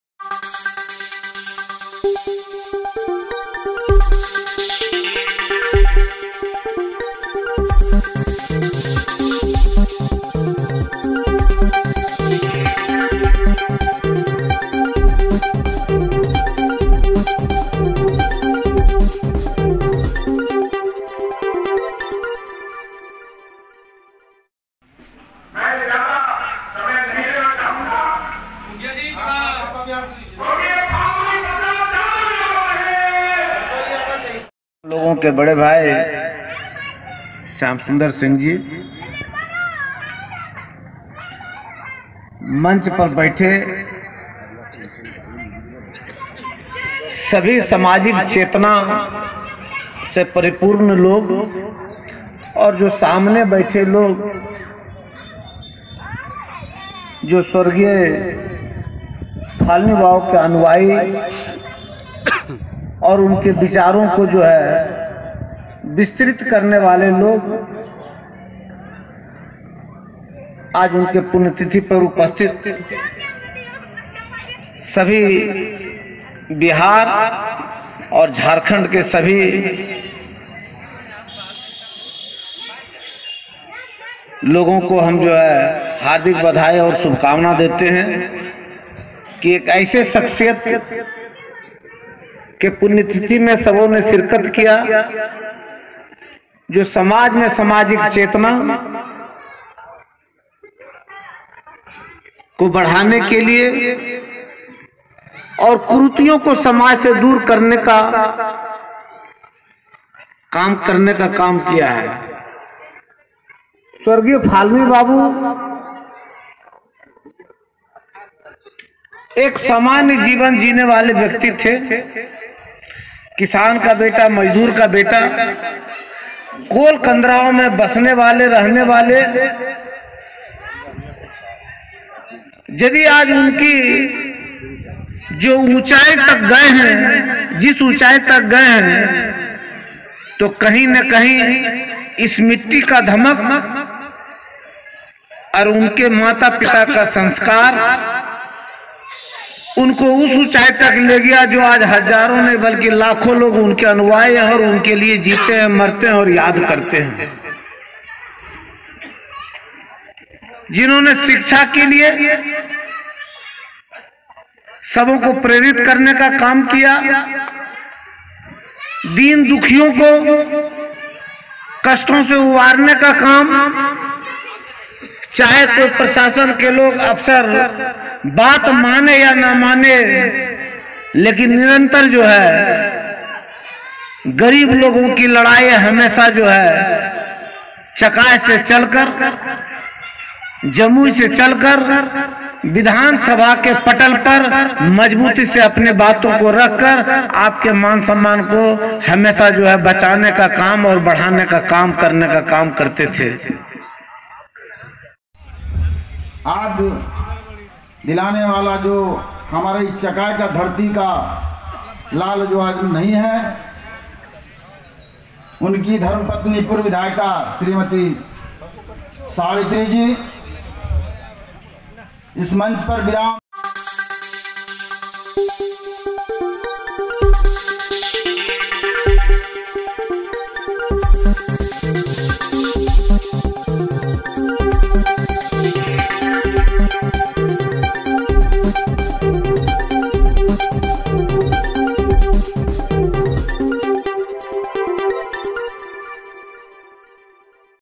स्व: फाल्गुनी प्रसाद यादव की आठवीं पुण्य तिथि संपन्न , समाधी स्थल पर किया गया श्रद्धांजलि अर्पित
:::::::::: पूर्व विधायक स्व फाल्गुनी प्रसाद यादव की आठवीं पुण्य तिथि संपन्न , समाधी स्थल पर मंत्री सहित अन्य लोगों ने किया श्रद्धांजलि अर्पित :::::::::
:::::::::::: JBJ NEWS 24 JAMUI BIHAR. ::::::::::::